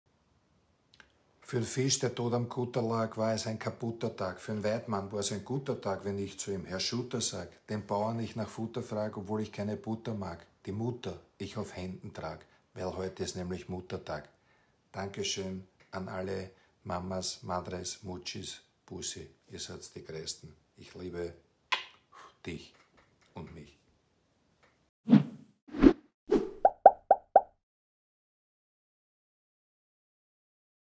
Muttertagsgedicht vom Goleador | Hans Krankl Parodie
Der Goleador sagt ein Muttertagsgedicht auf

Übrigens, mich gibt’s jetzt dann auch auf Insta, und TikTok, falls ihr Lust habt mir auch auf diesen Kanälen zu folgen, hier sind die Links: